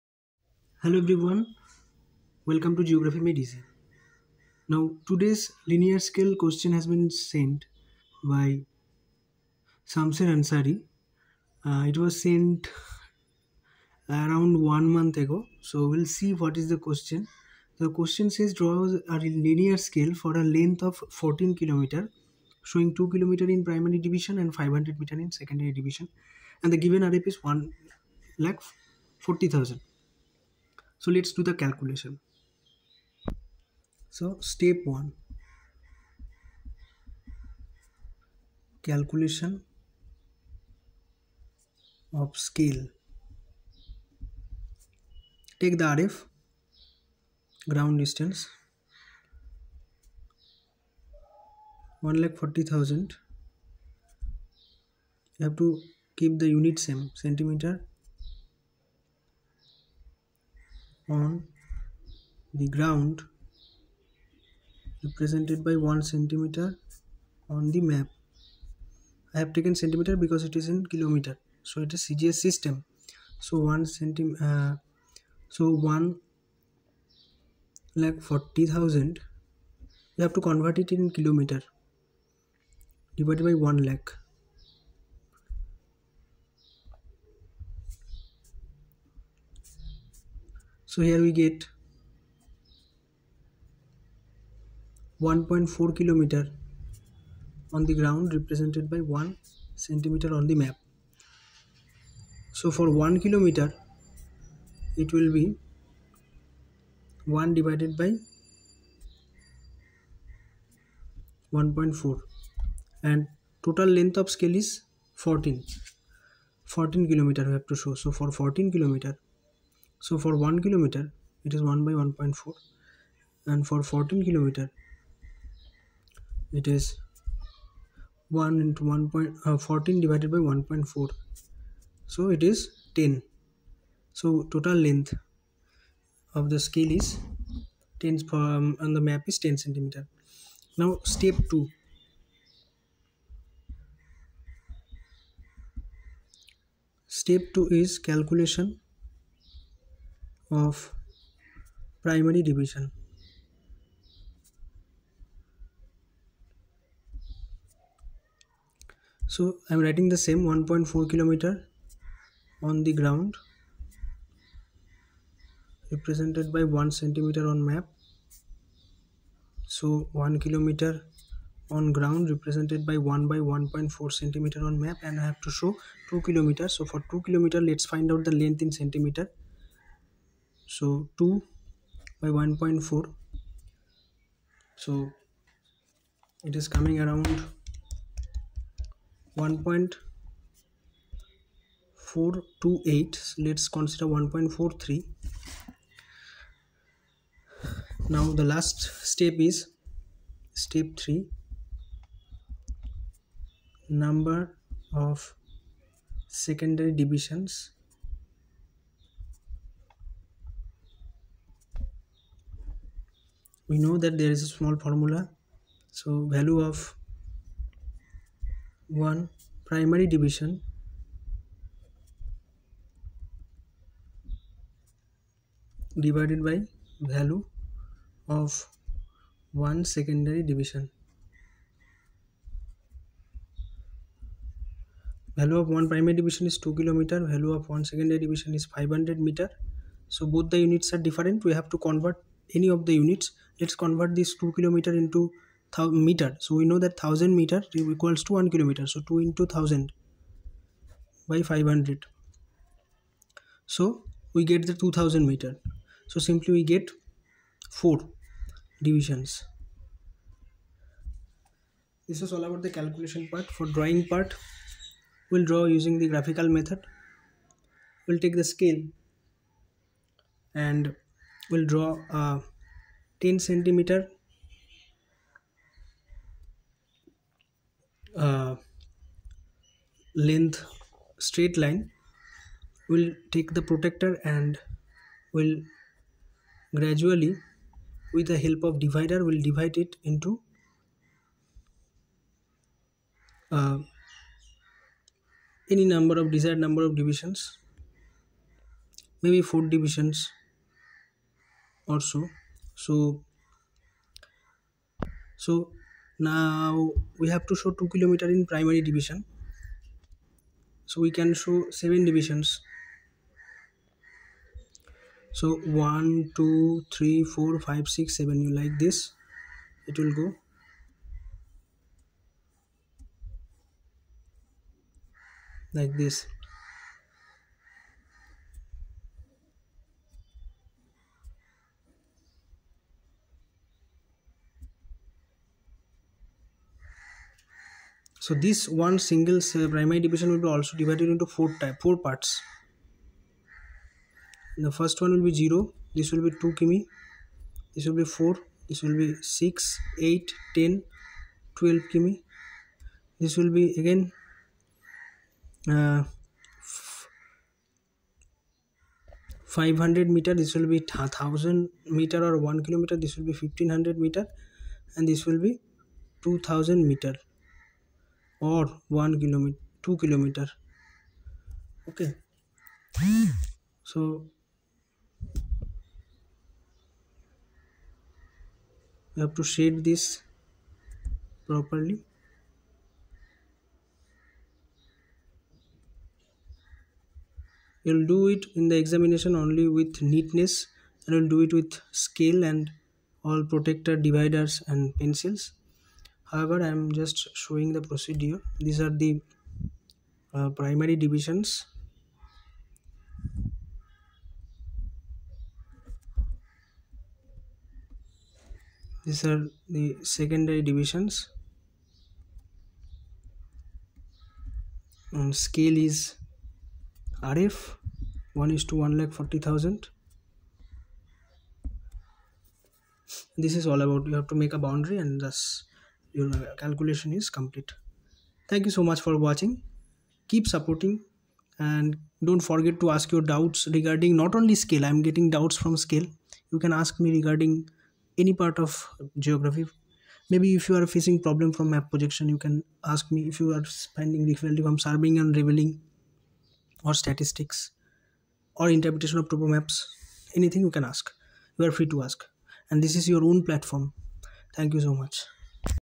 Draw a Linear Scale to Show 14km length, 2km in PD and 500 metre in SD | RF 1: 140000
or insta id: subhamroy_bikram
happy learning